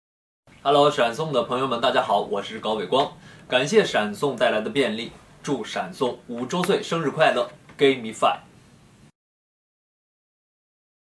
2019.3.19 闪送五周年Give me five 是我们爱的东华帝君，是@深情的高伟光 谢谢你的祝福呀，话说三生三世枕上书什么时候播出啊Vengo Gao Gao Wei Guang
高伟光[超话]# 是我们爱的东华帝君，是@深情的高伟光 谢谢你的祝福呀，话说三生三世枕上书什么时候播出啊（迷妹闪送日常催剧）闪送五周年Give me five#